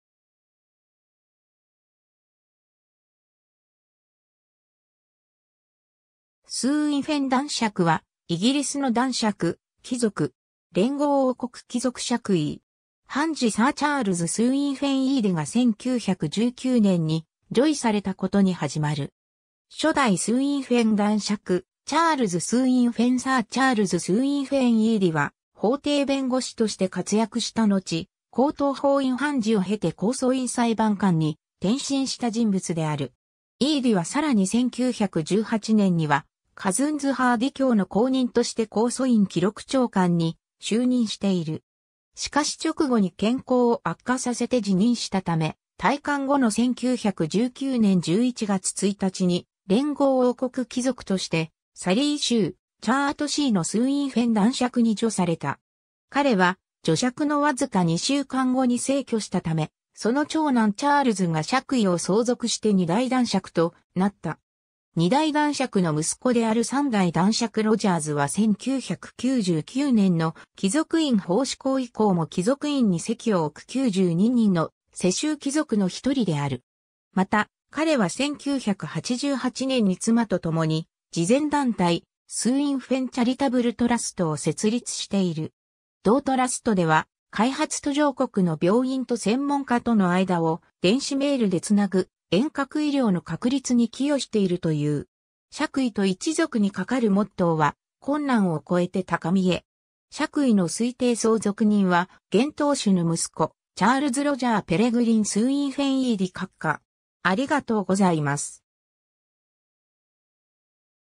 スウィンフェン男爵
スウィンフェン男爵（英語: Baron Swinfen）は、イギリスの男爵、貴族。
連合王国貴族爵位。
判事サー・チャールズ・スウィンフェン=イーディが1919年に叙位されたことに始まる。
 初代スウィンフェン男爵チャールズ・スウィンフェン サー・チャールズ・スウィンフェン=イーディ (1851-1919)は法廷弁護士として活躍したのち、高等法院判事を経て控訴院裁判官に転身した人物である。
イーディはさらに1918年にはカズンズ＝ハーディ卿の後任として控訴院記録長官に就任している。
しかし直後に健康を悪化させて辞任したため、退官後の1919年11月1日に連合王国貴族としてサリー州チャートシーのスウィンフェン男爵(Baron Swinfen of Chertsey in the County of Surrey)に叙された。
彼は叙爵のわずか2週間後に逝去したため、その長男チャールズ（1904–1977）が爵位を相続して2代男爵となった。
 2代男爵の息子である3代男爵ロジャーズ（1938-）は1999年の貴族院法施行以降も貴族院に籍を置く92人の世襲貴族のひとりである。
 また、彼は1988年に妻とともに慈善団体スウィンフェン・チャリタブル・トラストを設立している。
同トラストでは、開発途上国の病院と専門家との間を電子メールでつなぐ遠隔医療の確立に寄与しているという。
 爵位と一族にかかるモットーは、『困難を超えて高みへ(Per Ardua Ad Alta)』。
 爵位の推定相続人は現当主の息子チャールズ・ロジャー・ペレグリン・スウィンフェン=イーディ閣下（1971-）。